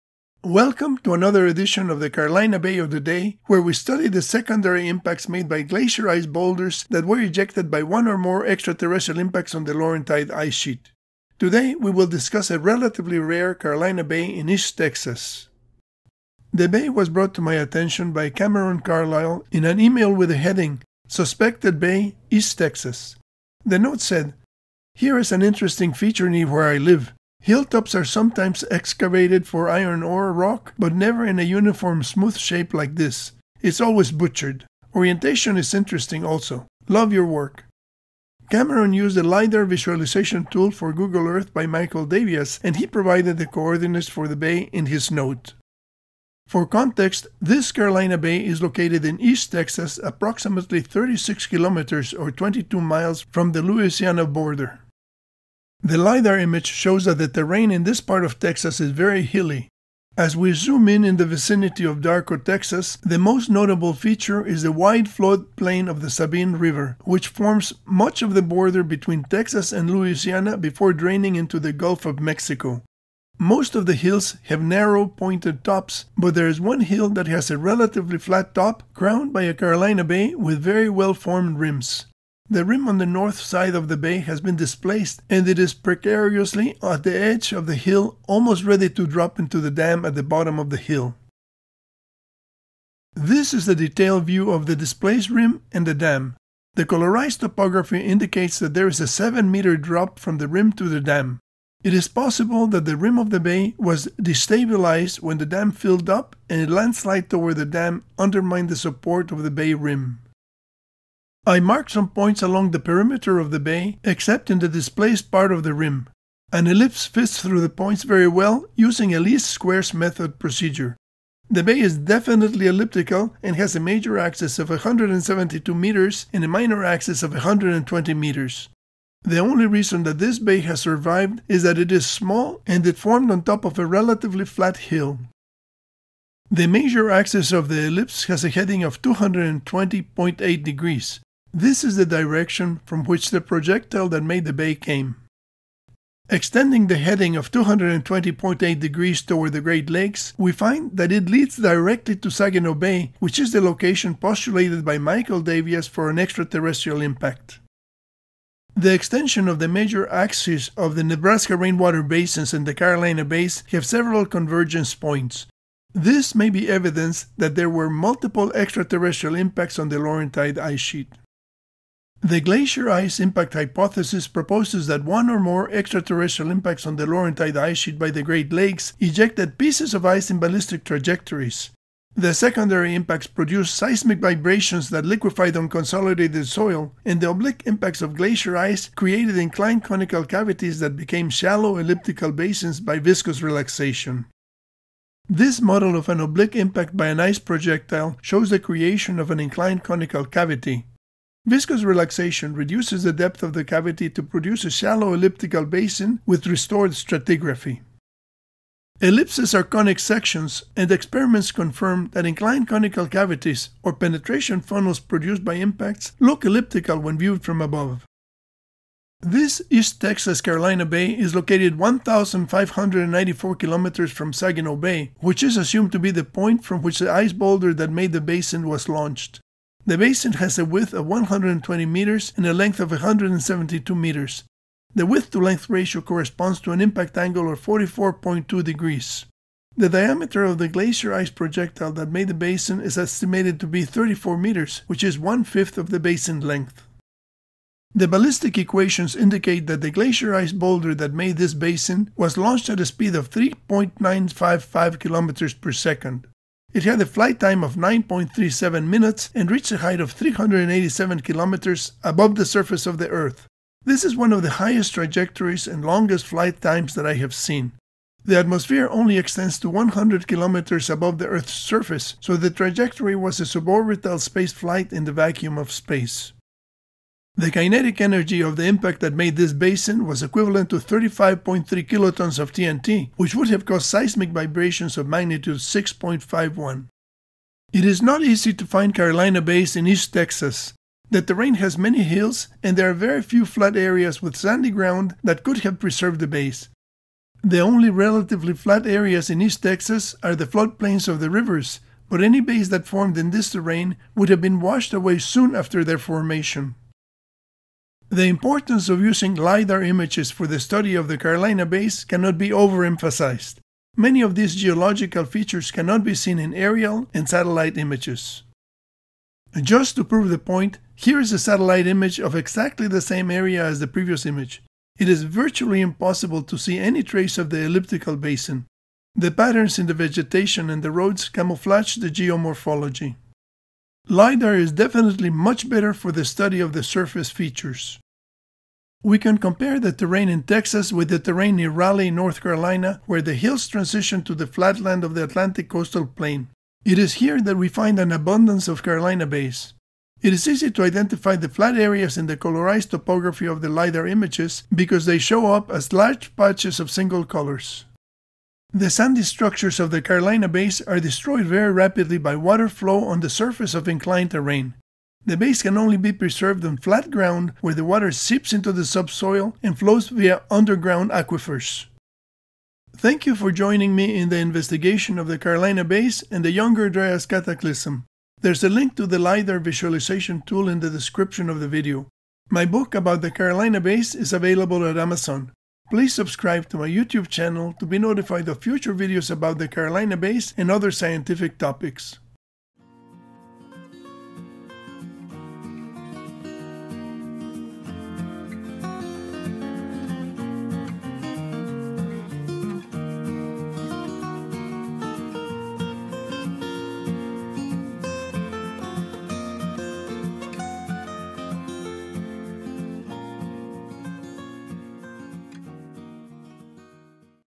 Carolina Bays in East Texas
It is not easy to find Carolina Bays in East Texas.  The terrain has many hills and there are very few flat areas with sandy ground that could have preserved the bays.

A. Zamora, A model for the geomorphology of the Carolina Bays, Geomorphology, 282, 209–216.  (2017), DOI 10.1016/j.geomorph.2017.01.019

Carolina Bay Survey. LiDAR Visualization Tool using Google Earth by Michael Davias.

Python programs for least squares fitting of ellipses.